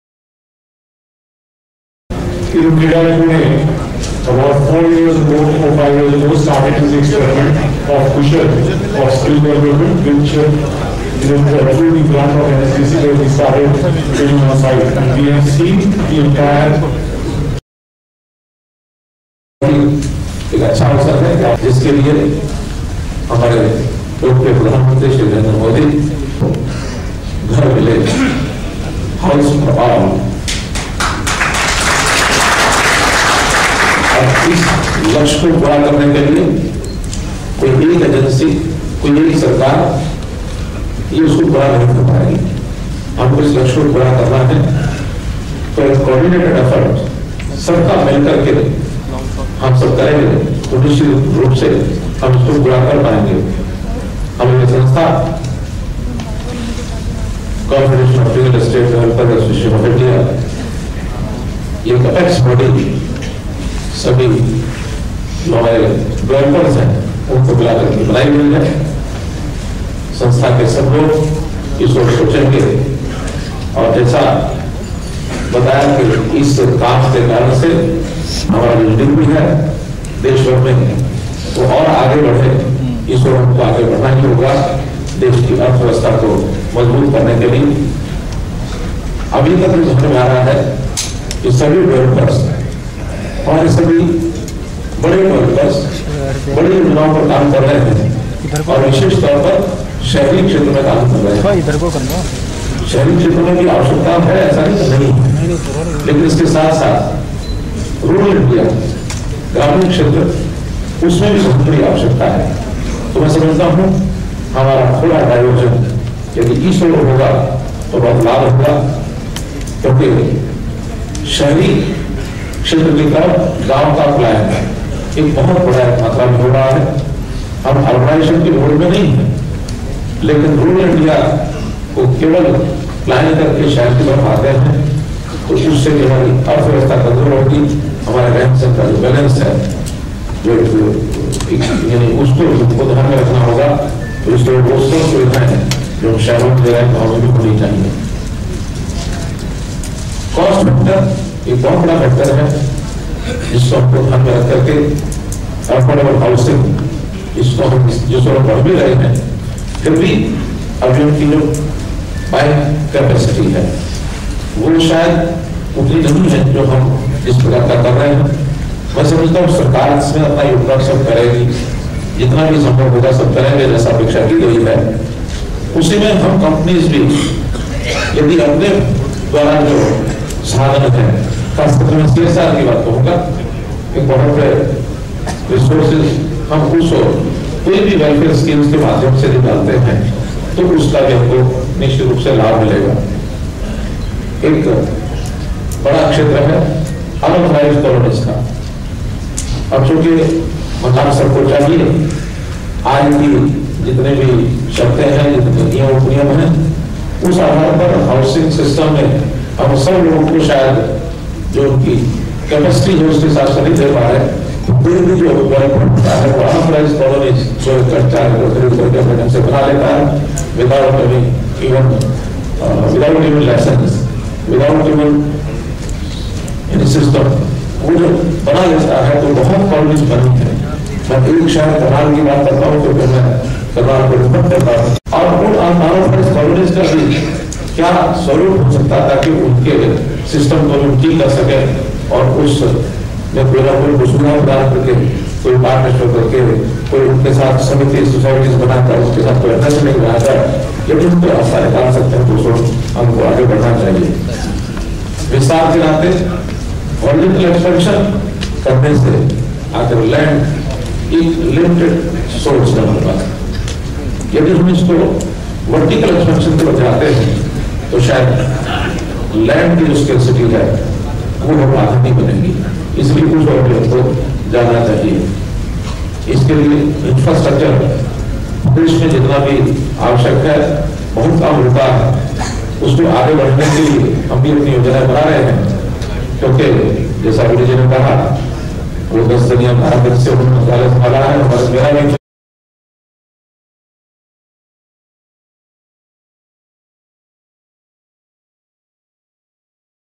CREDAI Conclave 2014 at New Delhi - Haryana CM Manohar Lal
Haryana Chief Minister Manohar Lal said that the State Government would take concrete steps to provide affordable houses to the people.
Coordinated efforts are needed to fulfill the aim of ‘houses for all’ and it is not possible for a single agency or the government to make availability of houses for one and all.
The Chief Minister was addressing the conference of Confederation of Real Estate Developers Association of India (CREDAI) organized at New Delhi today. He said that the State Government would consider tying up with CREDAI to provide housing facility to people.